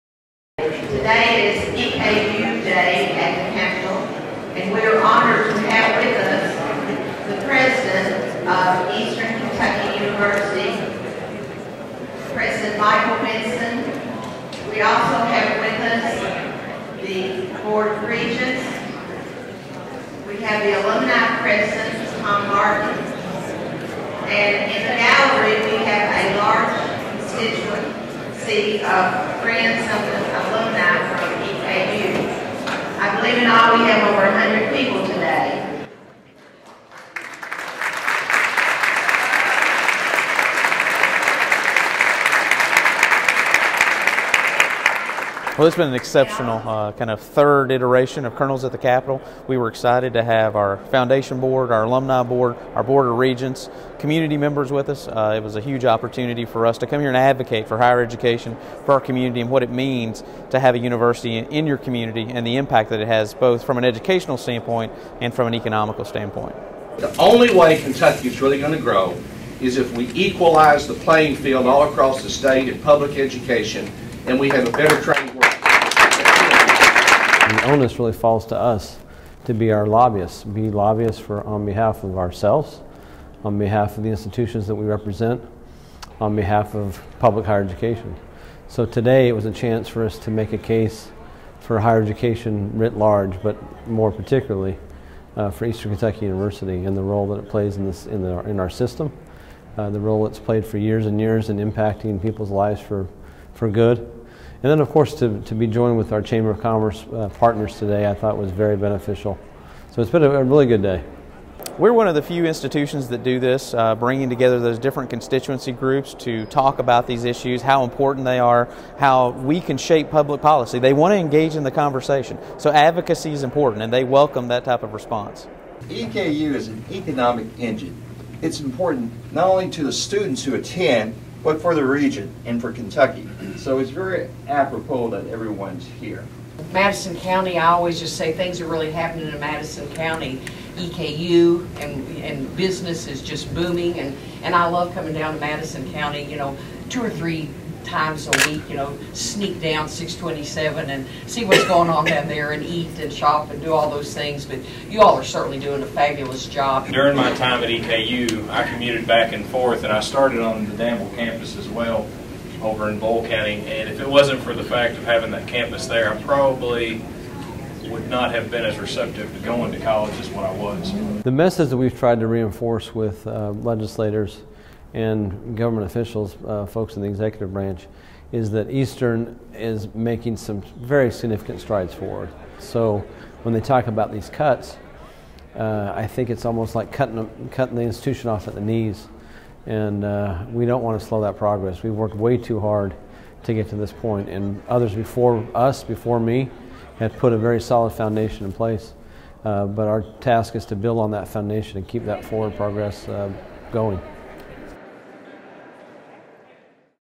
Colonels at the Capitol
EKU Staff & Faculty visited Frankfort to interact with members of the House of Representatives & the Senate for Colonels at the Capitol day.